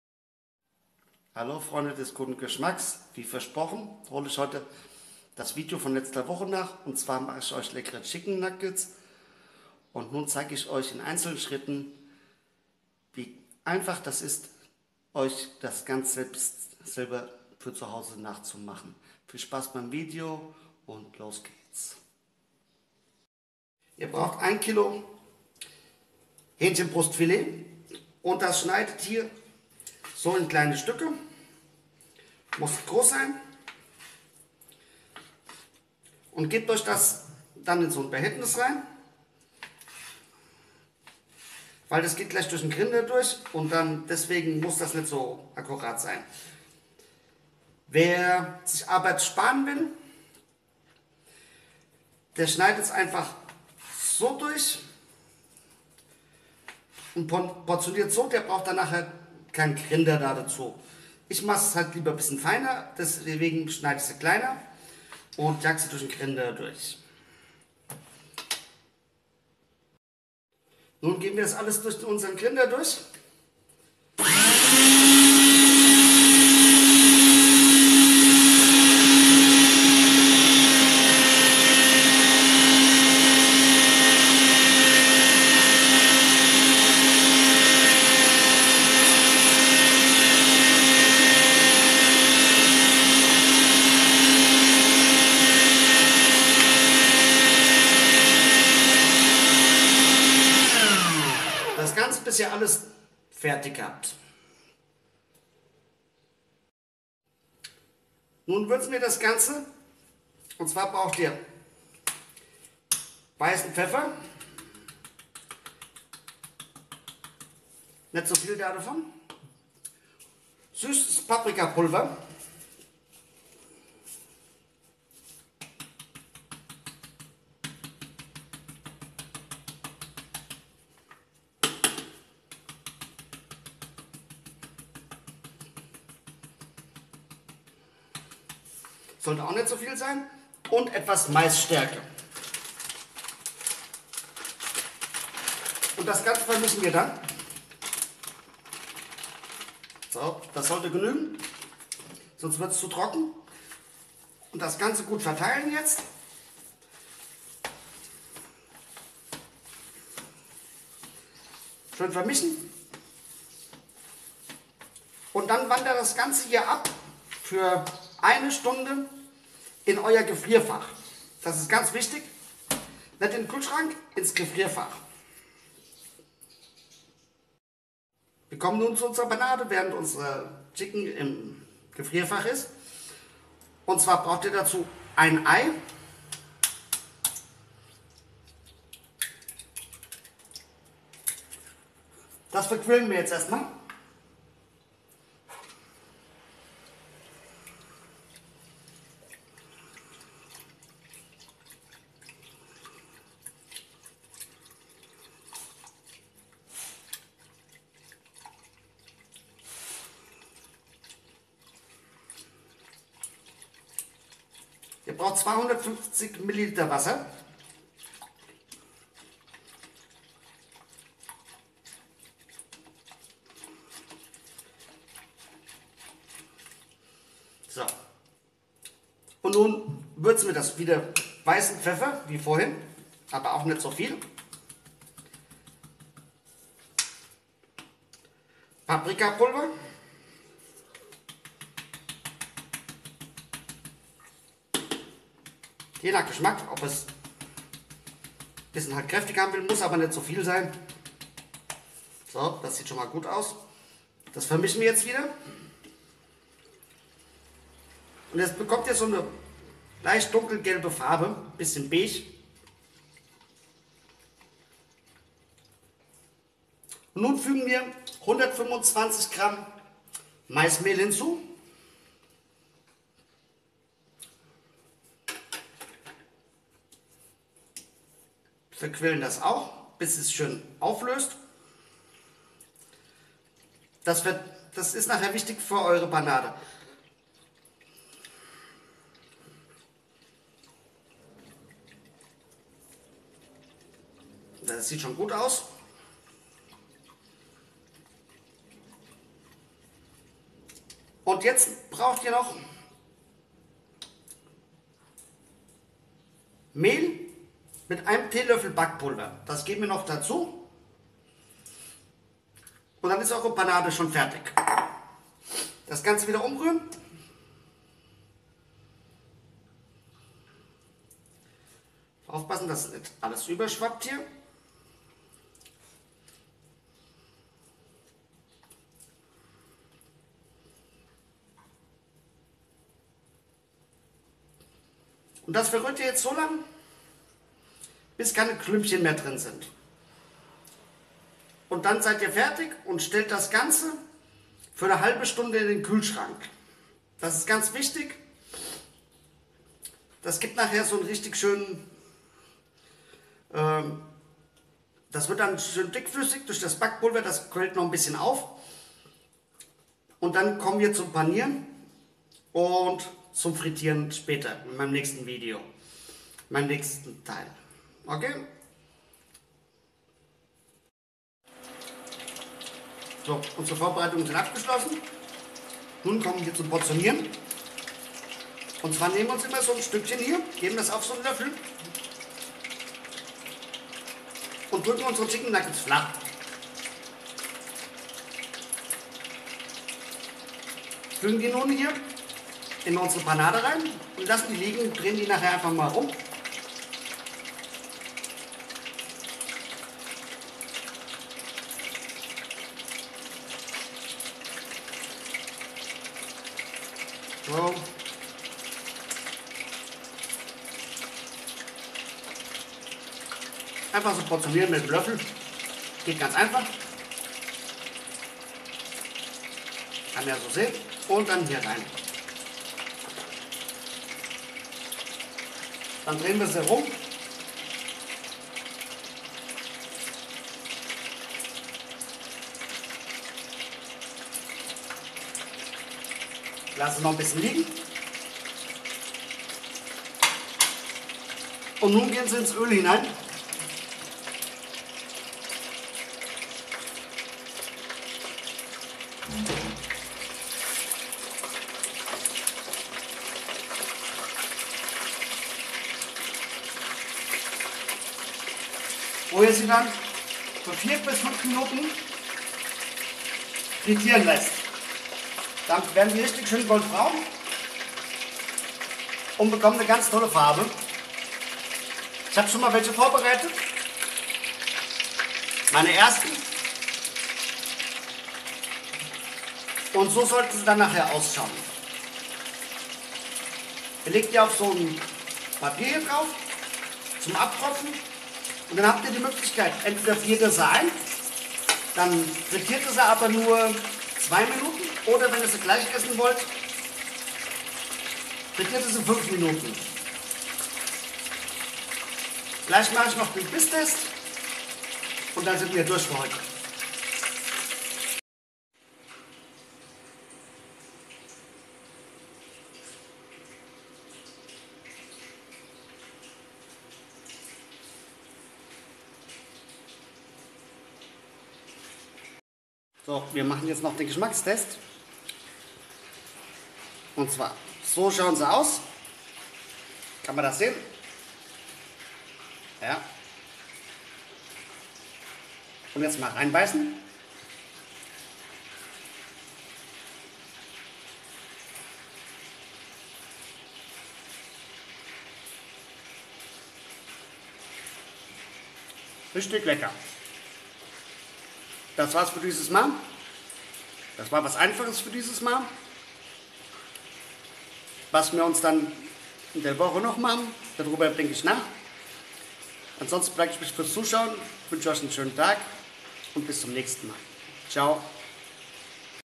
Selbstgemachte Chicken Nuggets
Chicken Nuggets ( Ca 40 Stück )
1 kg Hünchenbrust in kleine Stücke schneiden . Danach in den Gründer geben . Nun würzt ihr das Ganze mit etwas weißem Pfeffer , süßem Paprikapulver und etwas Maisstärke . Das ganze gut einkneten . Nun gebt ihr es für eine Stunde ins Gefrierfach .
Die Panade :
250 ml Wasser mit einem Ei verquirlen . Danach Paprikapulver und weißen Pfeffer hinzufügen . Nun 120 g Maisstärke hinzugeben und zu guter Letzt 200 g Mehl + 1 Tl Backpulver unterrühren . Das ganze geht dann für 30 Minuten in den Kühlschrank .
Nun portioniert ihr die Nuggets auf einem Esslöffel , gebt Sie in die Panade und Frietierte Sie für etwa 5 Minuten . Wer seine Nuggets einfrieren möchte nur zwei Minuten fritieren .Am Schluß auf einen Teller mit ausgelegtem Papier zum Abtropfen legen .